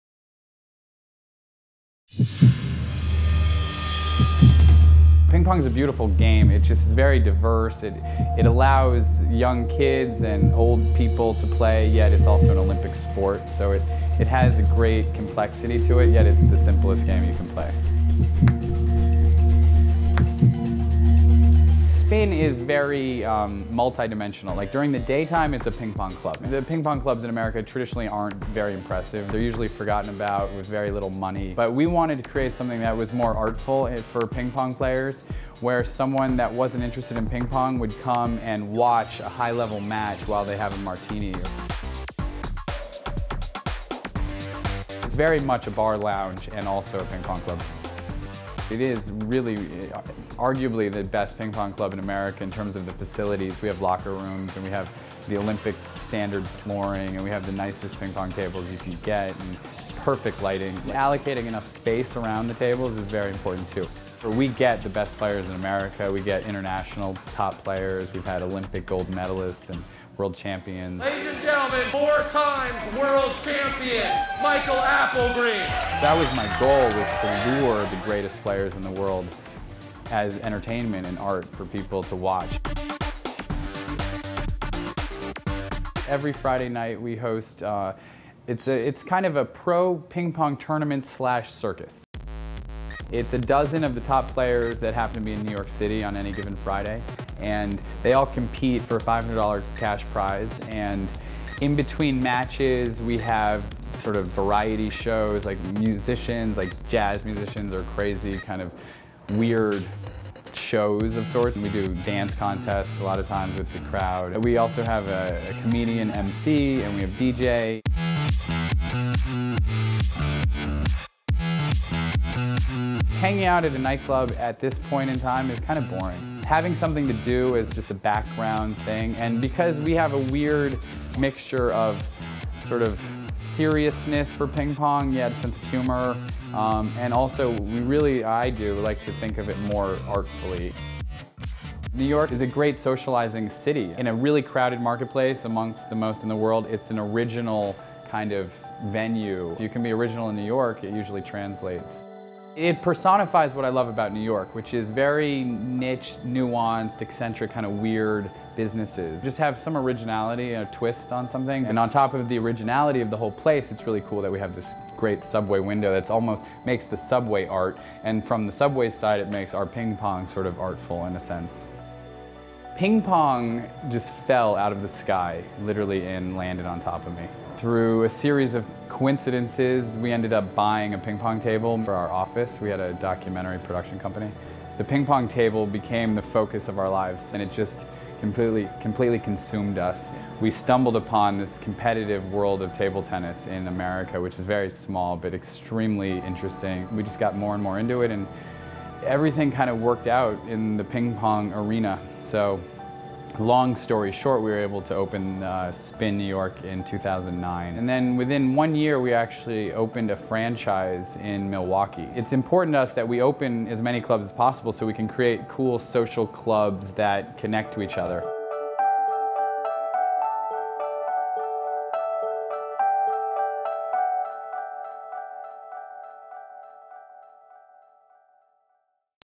SPiN
Part bar, part club, part restaurant and part ping-pong hall, SPiN defies definition. The trendy venue in midtown Manhattan is loved by native New Yorkers, tourists and celebrities alike - Mike Myers, Scarlett Johansson, Susan Sarandon and Jimmy Fallon can regularly be seen battling it out at the ping pong tables. Founder Jonathan Bricklin tells Crane.tv why balls are his business and why SPiN is the best off-Broadway show in town.